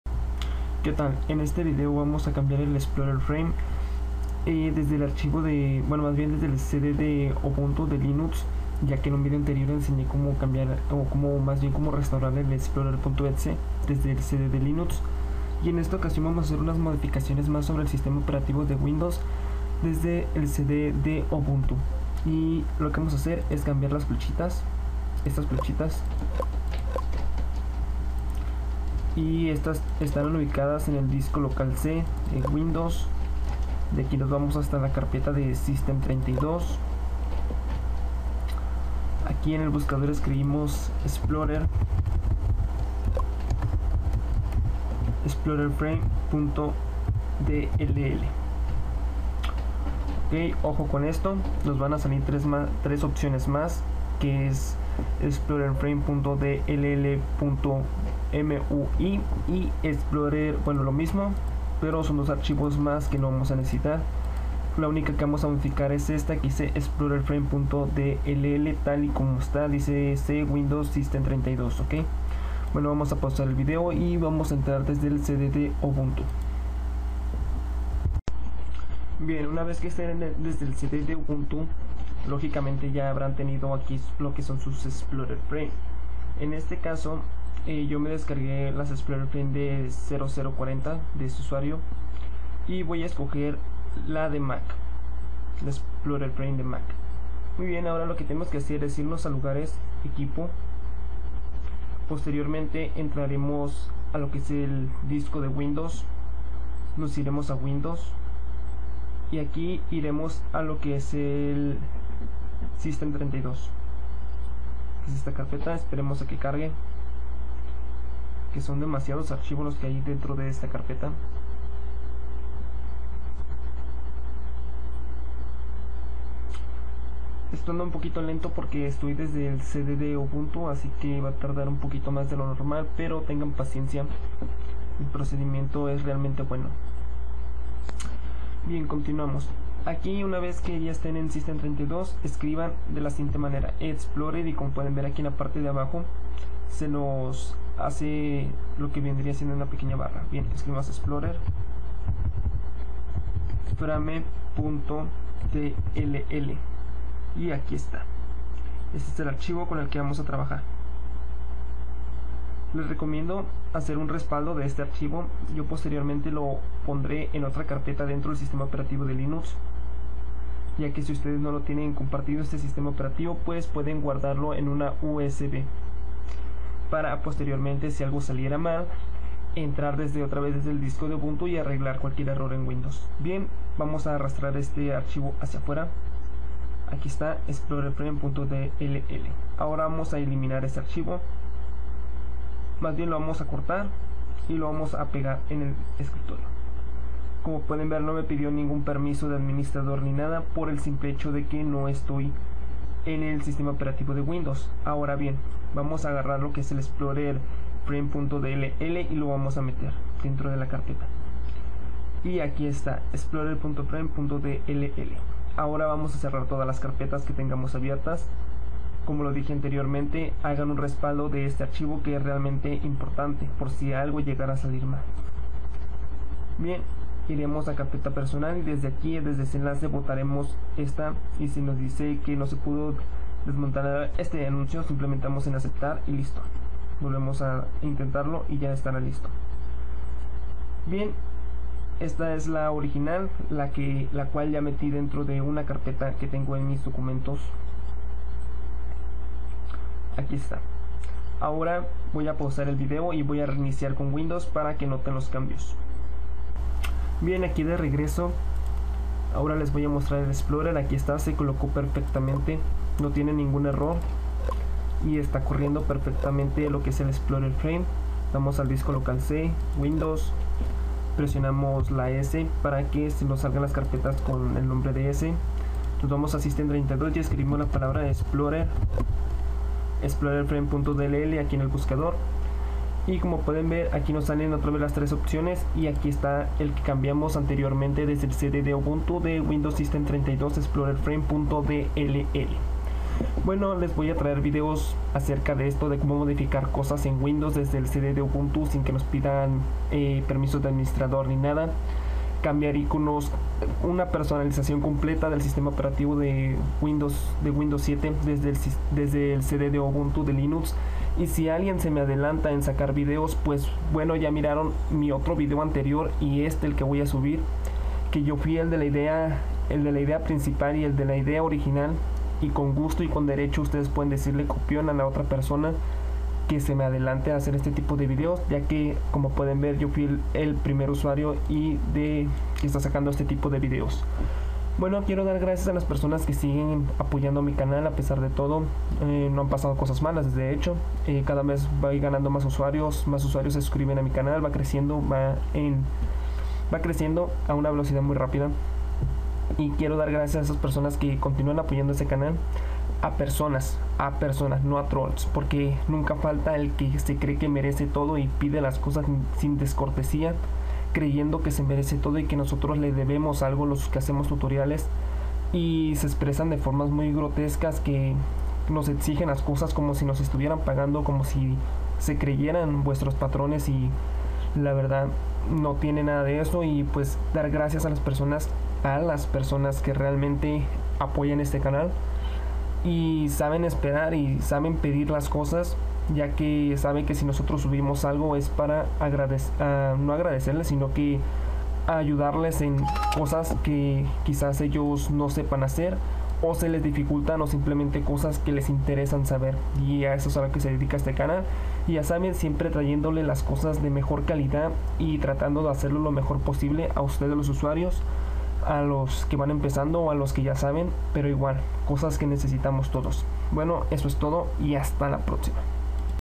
2020 - Como cambiar el explorerframe.dll de windows 10 sin permisos de administrador
sencillo, facil, rapido, sin errores, simplemente cool =D
RECUERDA QUE COMENTAR MOTIVA A SEGUIR APORTANDO!!
SIEMPRE CON CALIDAD NO PACKS DE WALL !!! AHAHAH